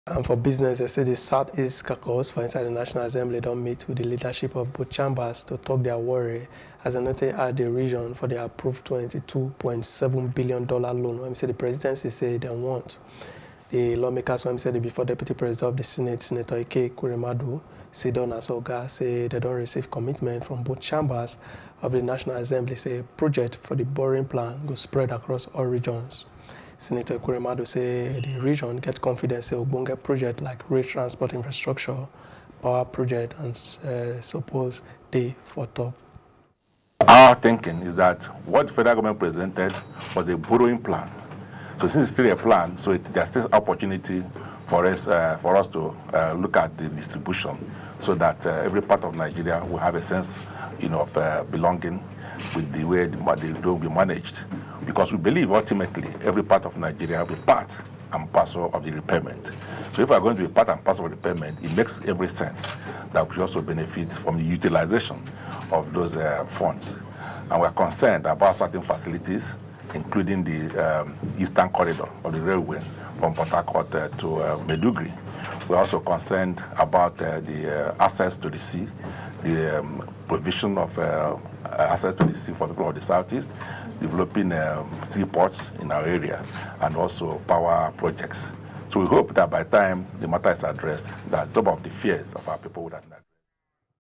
N22.7 BN Loan: Reps From South-East Tok Dia Mind As Dem Comot The Region
The South-East Caucus for the National Assembly don meet with the leadership of both Chambers to tok their worry as dem nor take add de region for the approved twenty-two point seven Billion dollar loan wen the Presidency say dem want.